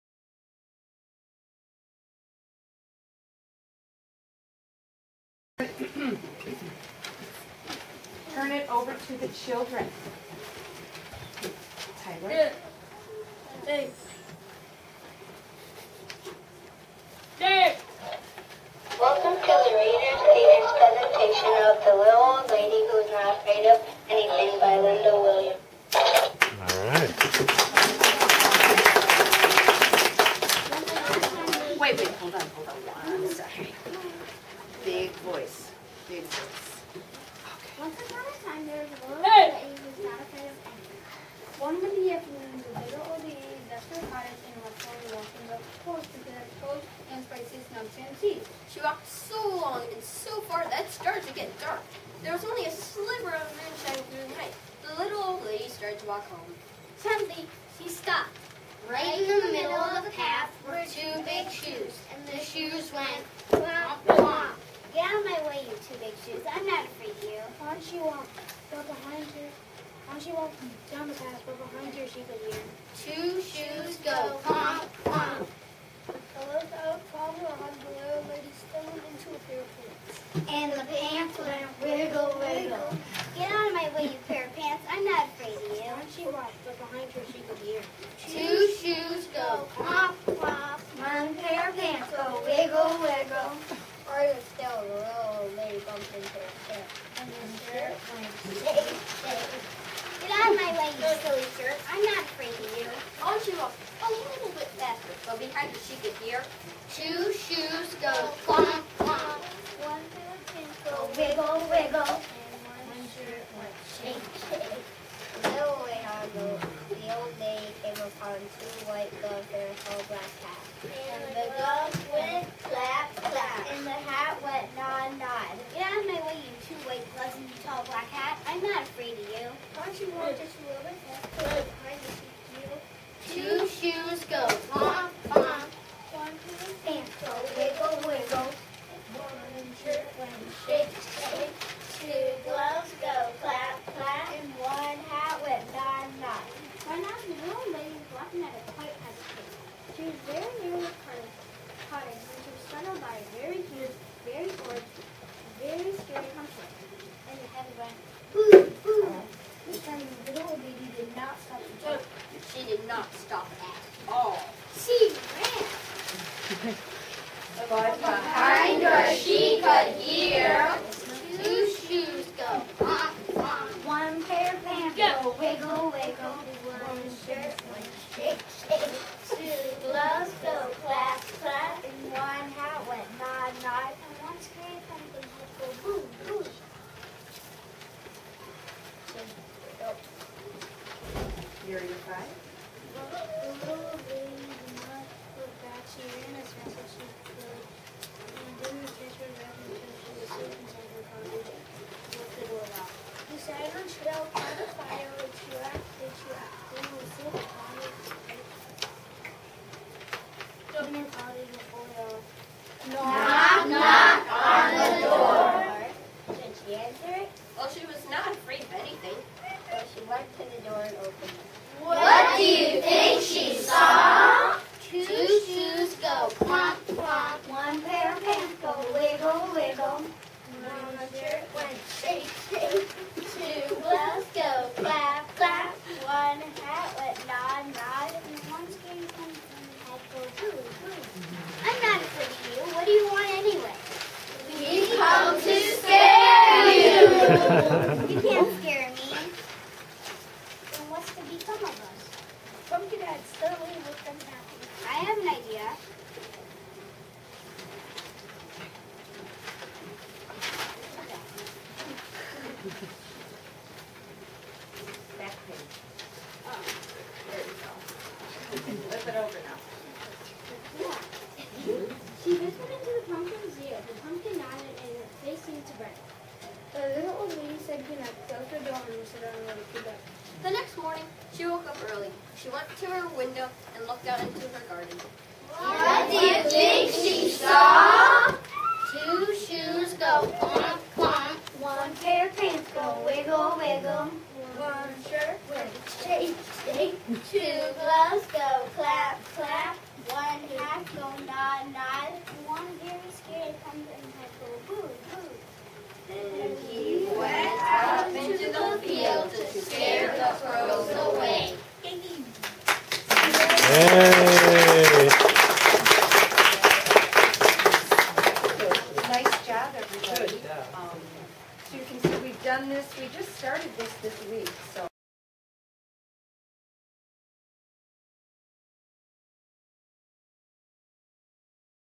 Reading play
Mr's Glennon's 3rd grade class doing a 'reading' play